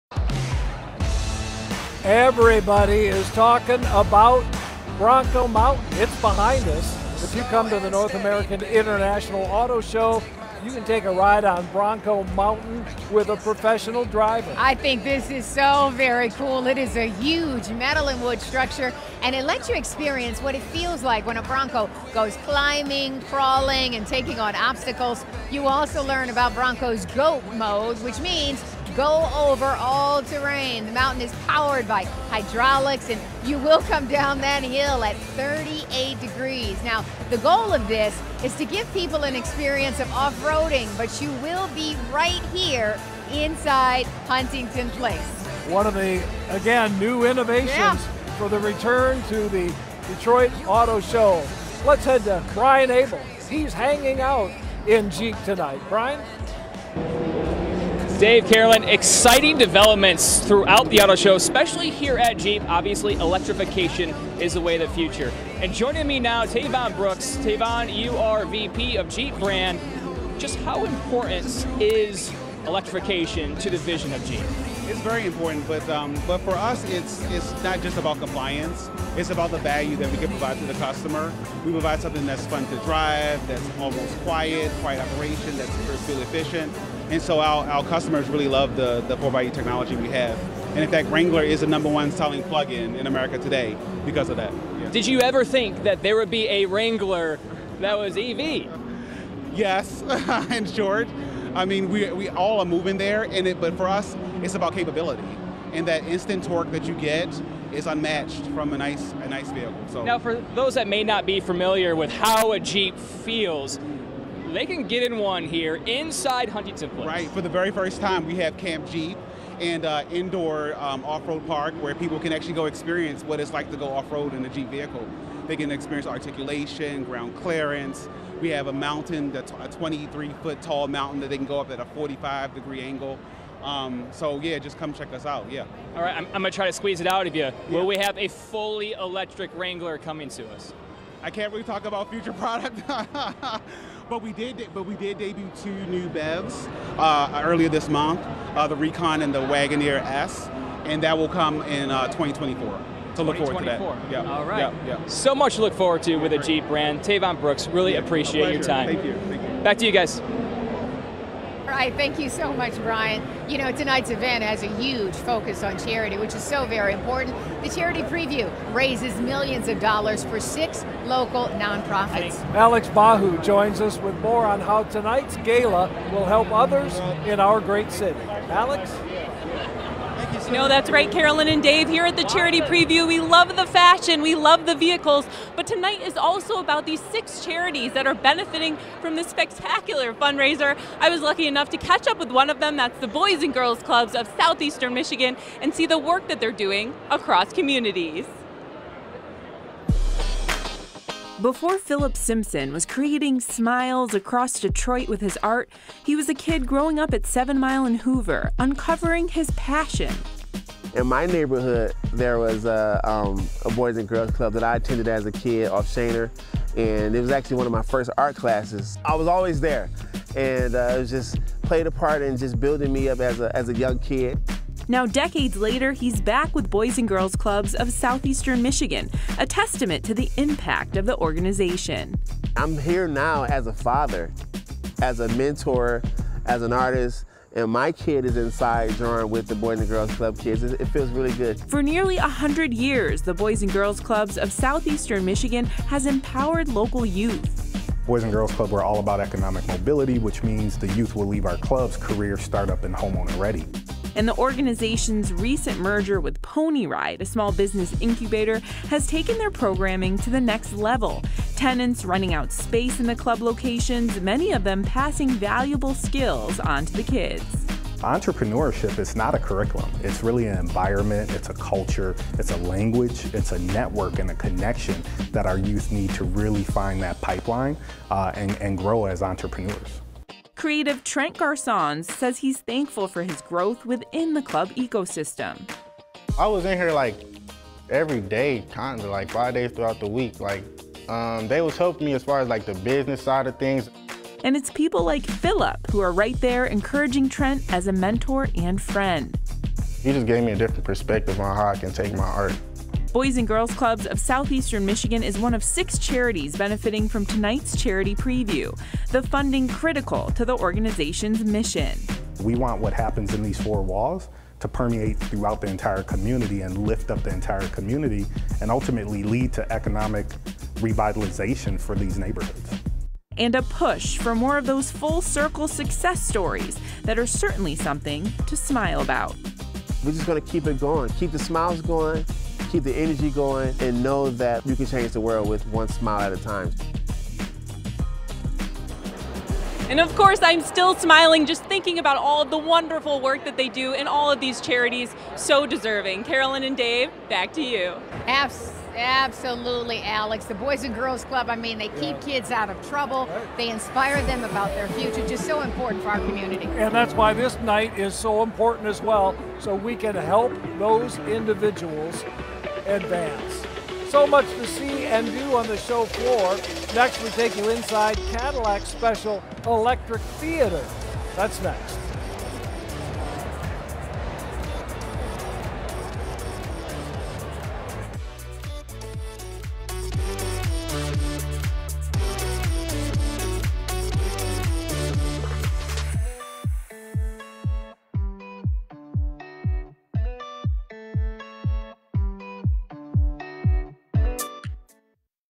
2022 Red Carpet Charity Preview - Segment 3
WXYZ-TV takes you inside the 2022 North American International Auto Show's Red Carpet Charity Preview.